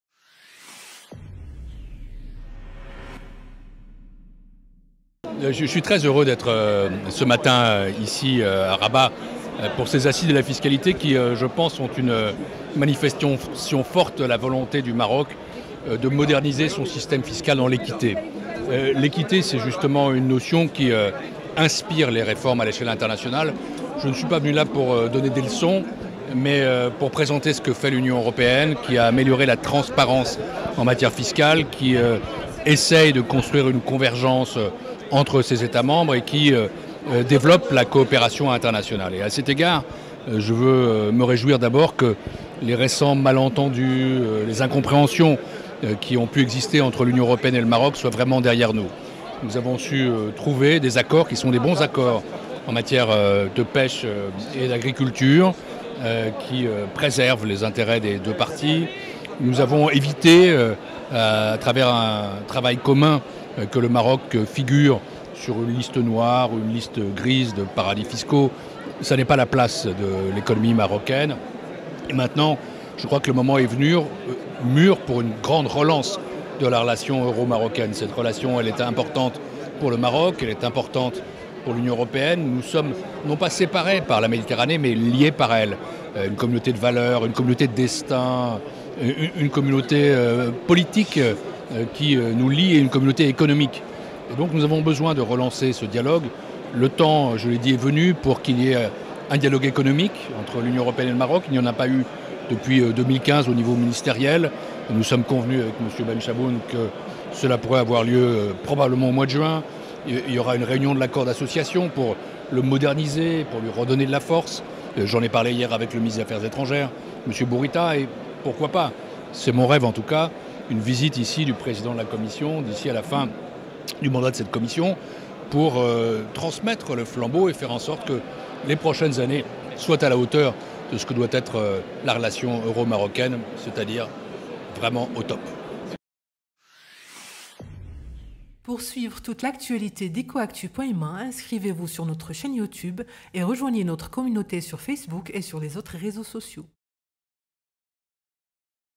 [WEBTV] Pierre Moscovici appelle le Maroc à lancer la réflexion sur la taxation du numérique
Après avoir rappelé les dessous du dialogue de plus en plus fructueux entre le Maroc et l'Union Européenne, Pierre Moscovici Commissaire européen aux Affaires économiques et monétaires, à la Fiscalité et à l’Union douanière explique que la réforme fiscale occupe une place de choix dans la redéfinition de la relation Europe-Maroc. Le leitmotiv est d'aboutir à une équité fiscale voire une bonne gouvernance fiscale.